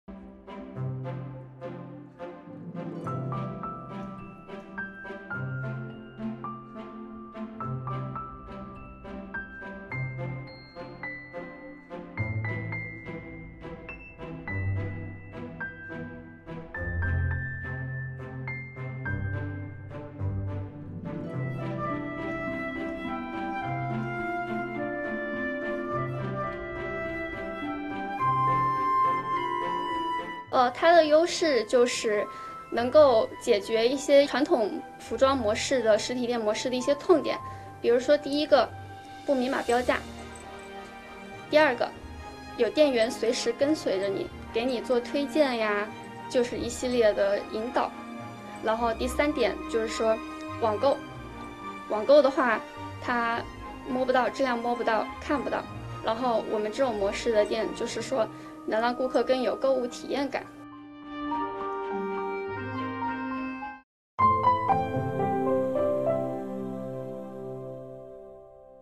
Self-service clothing store gains popularity in China
A self-service clothing store gains popularity in Central China’s Wuhan City, bringing thousands of dollars in monthly profits.

The store covers about thirty square meters and has seven rows of clothes, which are displayed in an orderly manner according to categories, such as dresses, coats, trousers and more. Special reminders hang on the wall, such as “turn on/ off the lights” and “bags available”. Customers can scan the QR code for checkout.

Wang Xuqing, a graduate student majoring in business administration, opened this self-help clothing store in early 2022. The shop owner only spends about one hour replenishing, ironing, and disinfecting every day, saving time and labor costs greatly.

Self-service retailing is sweeping China. There are currently over 270 self-service stores in Wuhan City, including 24-hour studies, shared teahouses, dance studios, billiard halls and more, according to Meituan.com, a tech-driven retail company in China.